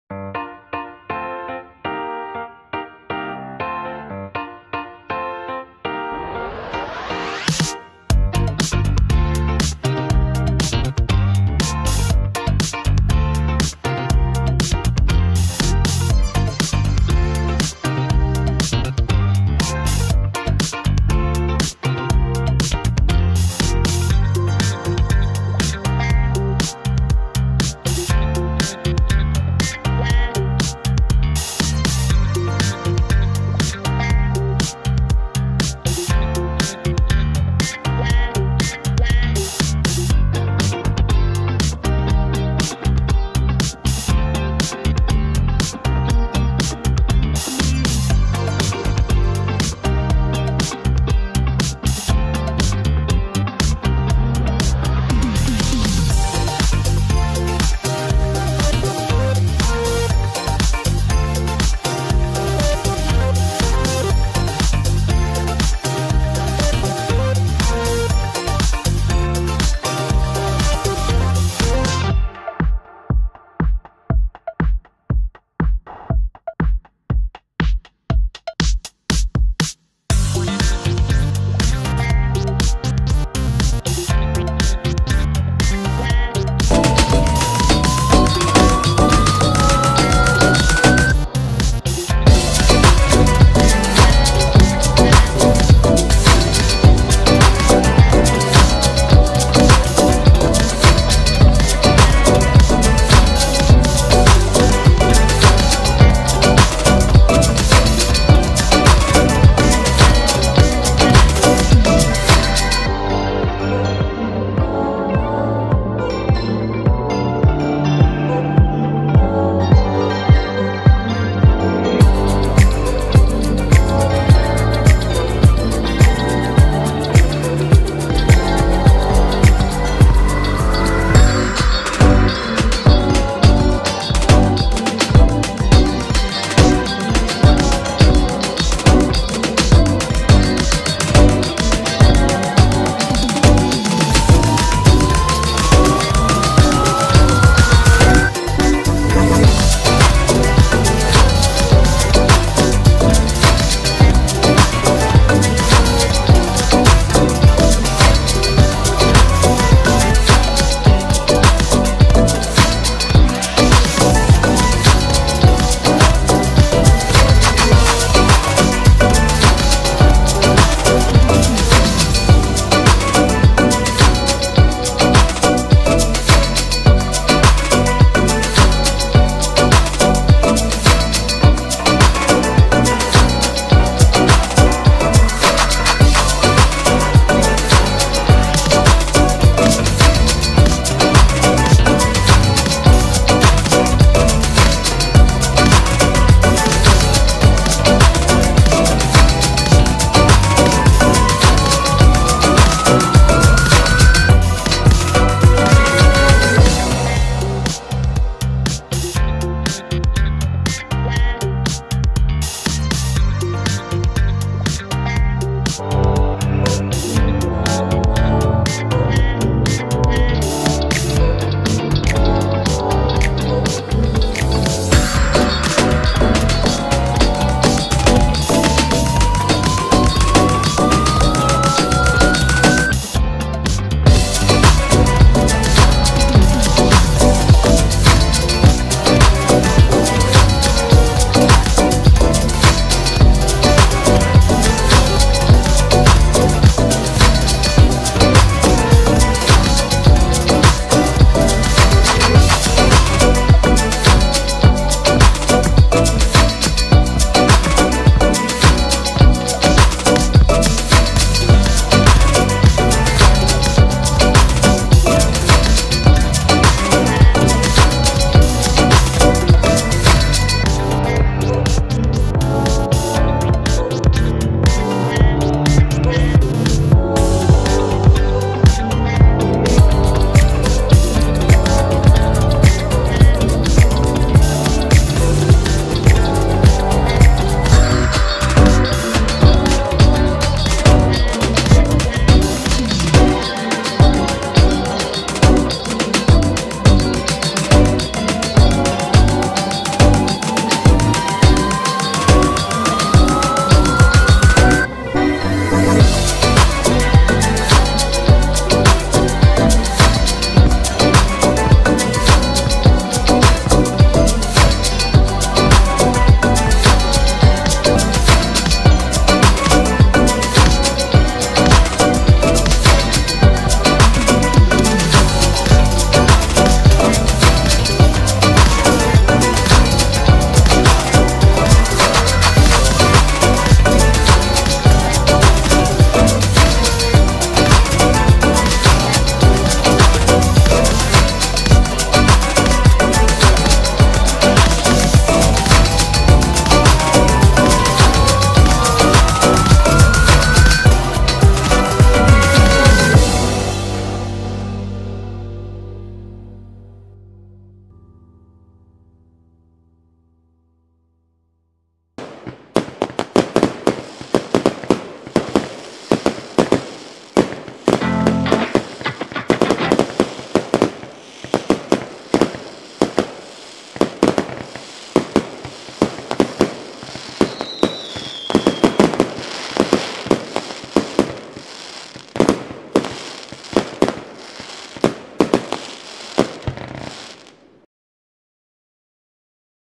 The many statues of Sir William Penn Made with Clipchamp
Here is a great look at Philadelphia's founding father Sir William Penn.